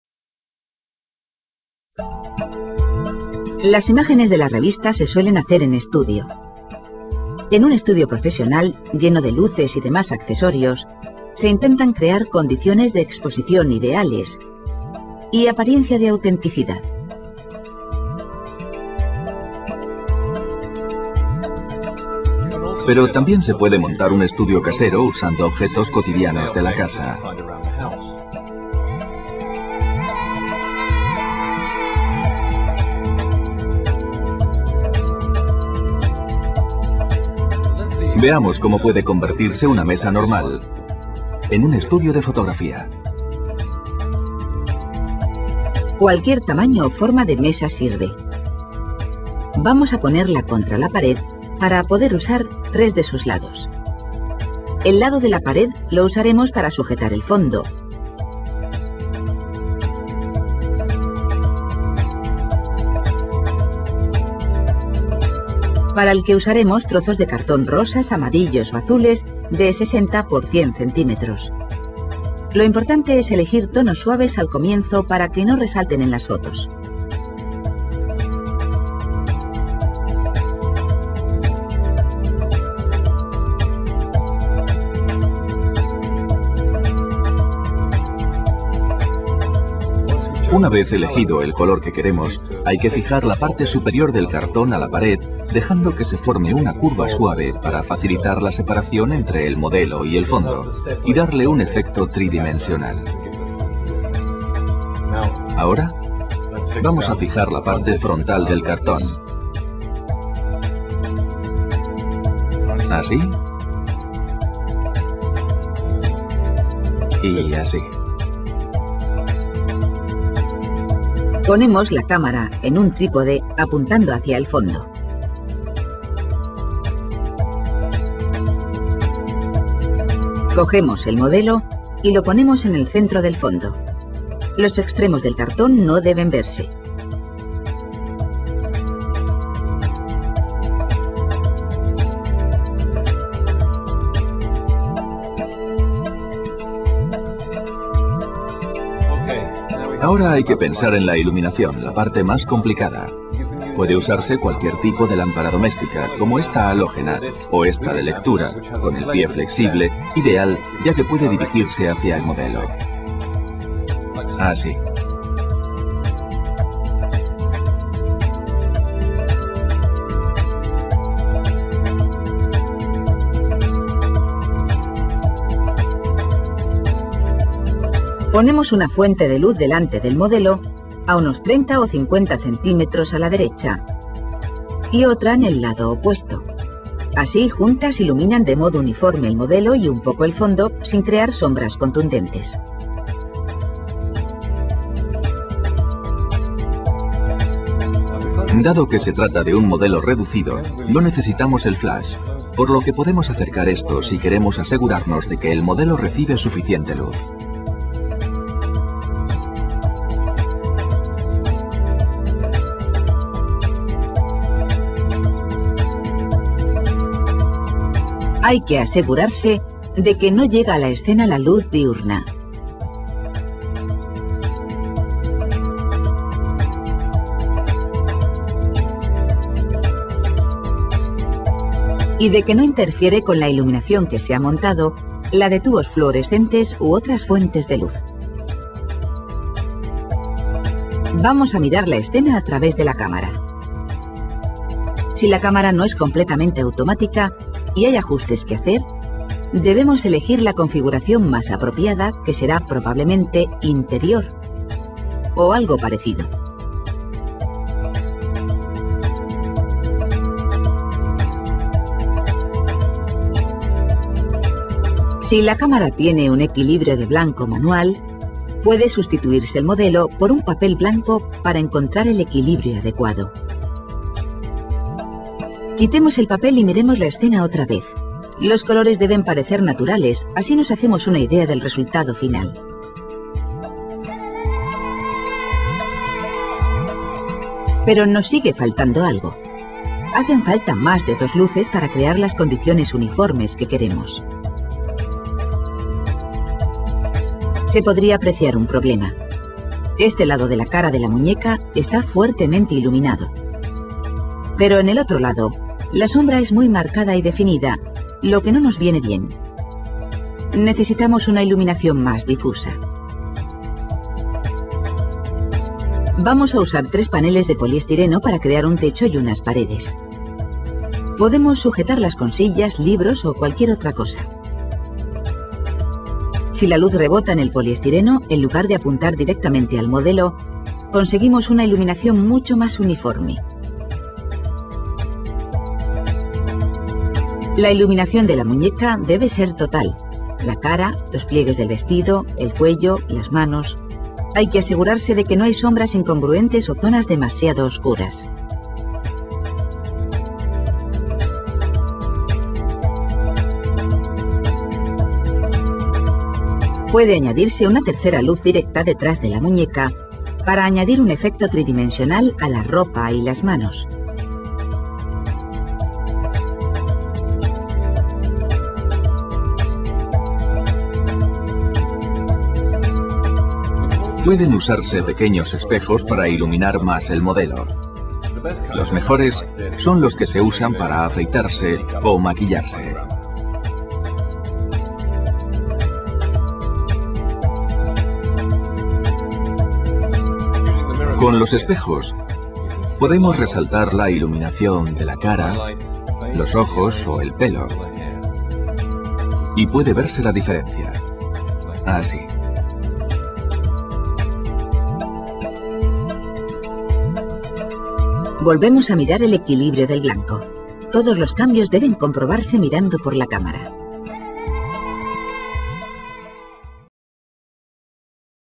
Crea un estudio casero para fotografia.
Este es un video de como crear un estudio casero en fotografia , es bien completo.

*SE HAN DESACTIVADO LOS COMENTARIOS POR SALIDA A VACACIONES (Por todo Febrero)*